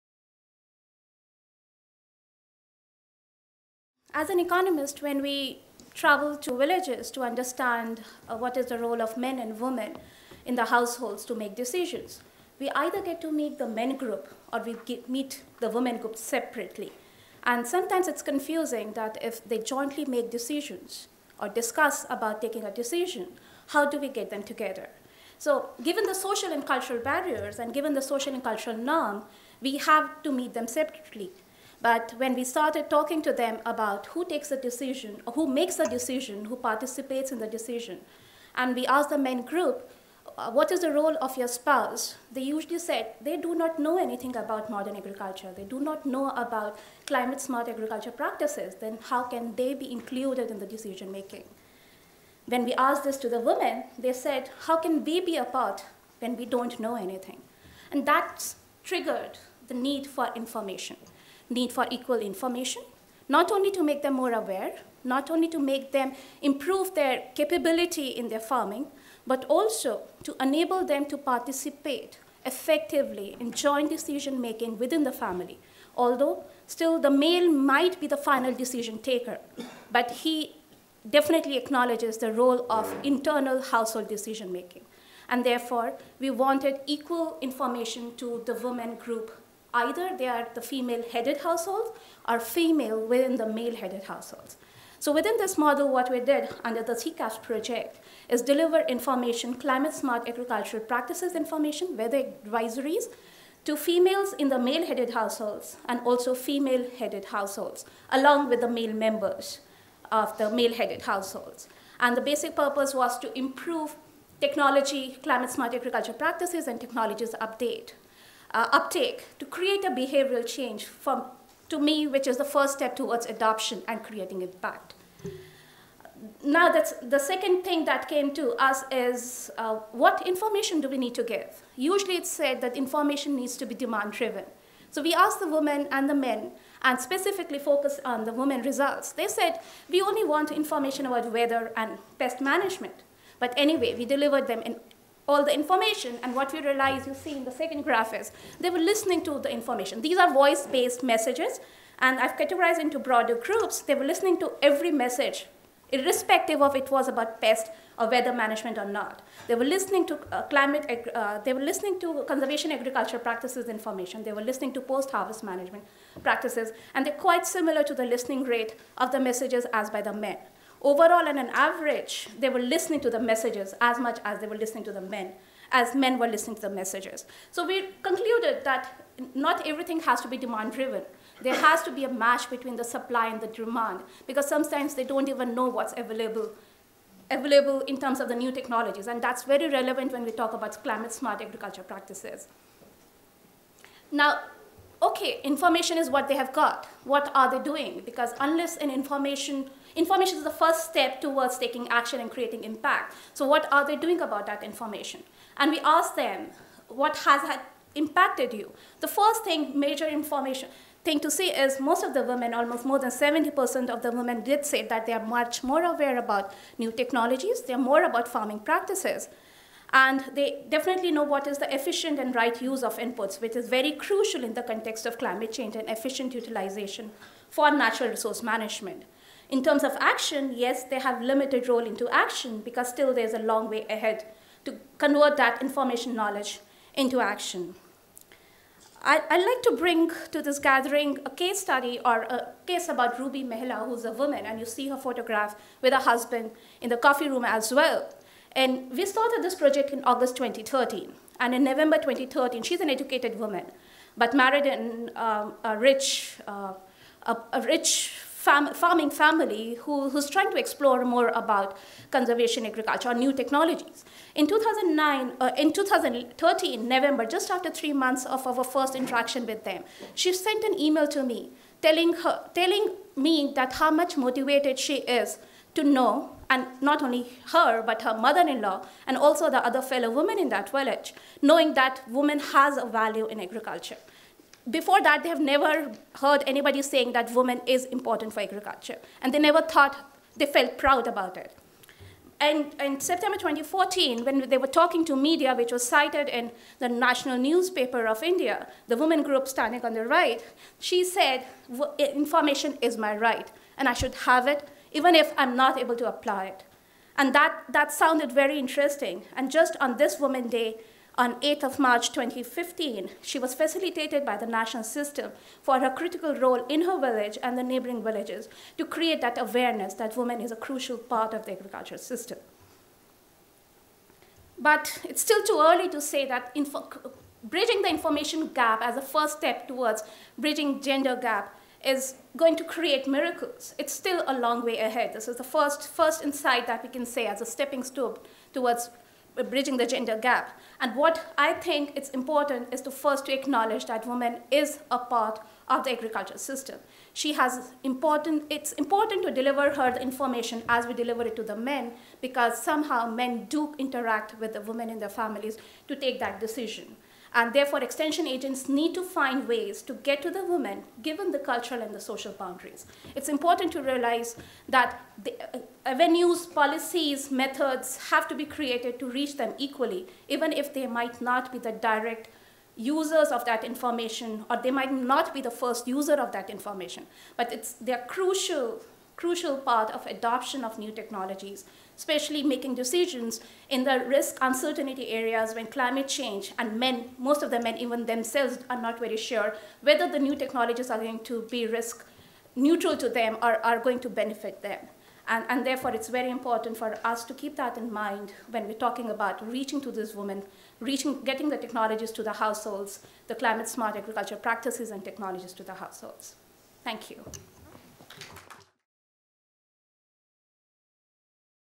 Surabhi Mittal on Mobile phone-enabled agro-advisory services
Mobile phone-enabled agro-advisory services: a means to empower women farmers

Presentation by Surabhi Mittal from CIMMYT at the seminar on Closing the gender gap in farming under climate change held 19 March 2015 in Paris.